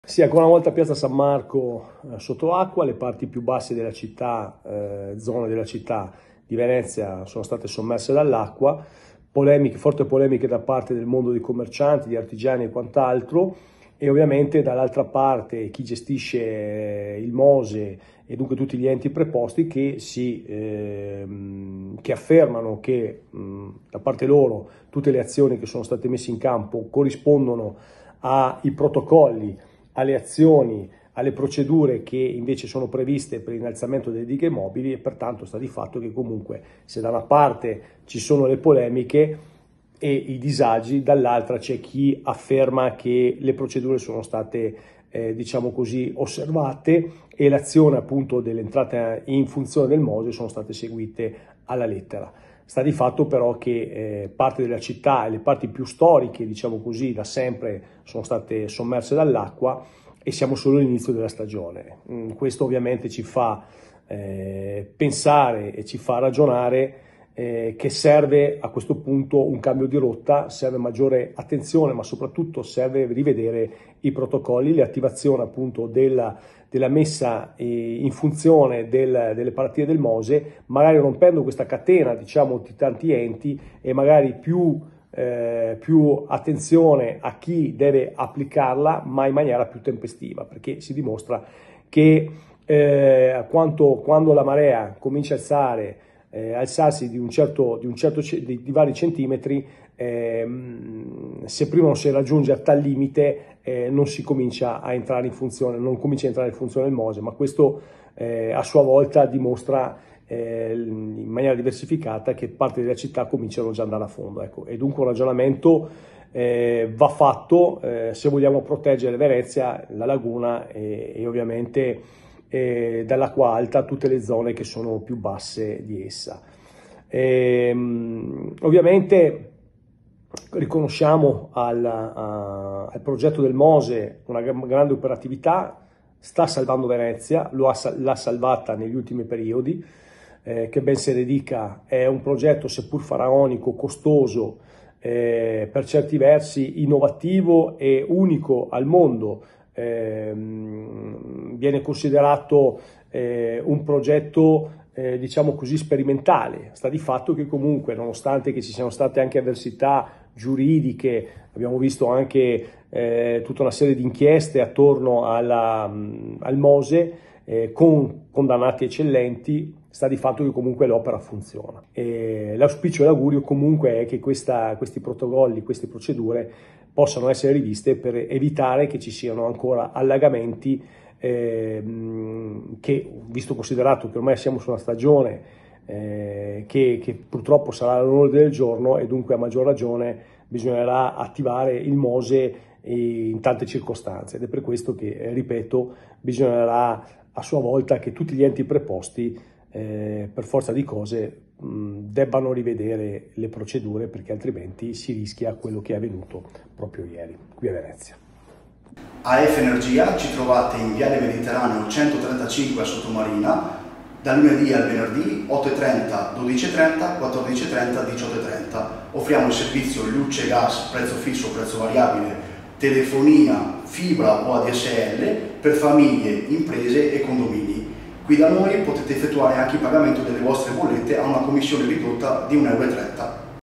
MOSE, DOLFIN LEGA LV: “URGENZA DI RIVEDERE LE PROCEDURE PER PROTEGGERE VENEZIA”
"Piazza San Marco è stata nuovamente allagata nonostante l'attivazione del Mose. È evidente l’urgenza di rivedere le attuali procedure di gestione del sistema di barriere mobili." Così si esprime Marco Dolfin, consigliere regionale e Capo Dipartimento per la Lega in Veneto, che sottolinea la necessità di un intervento immediato dopo gli eventi accaduti ieri.

Dolfin continua: "Il protocollo attuale, che prevede la consultazione di più enti prima di attivare il Mose, si è rivelato inefficace nel proteggere le aree più vulnerabili della città, come Piazza San Marco e altre zone critiche. Il Mose, progettato per difendere Venezia dall'acqua alta, deve essere attivato più rapidamente e adattato meglio alle condizioni reali."

Il consigliere ha sottolineato che la piazza è stata sommersa ieri a causa del ritardo nell'attivazione del sistema: "Le barriere sono state sollevate solo quando la marea aveva già raggiunto i 90 cm a Punta della Salute, causando disagi significativi per residenti e attività commerciali. È evidente che l’attuale gestione non può proseguire in questo modo."

Dolfin evidenzia inoltre che l'incompleta realizzazione dei lavori di innalzamento e impermeabilizzazione di Piazza San Marco impone una revisione immediata delle procedure: "Serve un sollevamento del Mose più frequente, anche con maree inferiori alle soglie attuali, per evitare ulteriori danni strutturali ed economici che gravano sia sui cittadini che sull’amministrazione."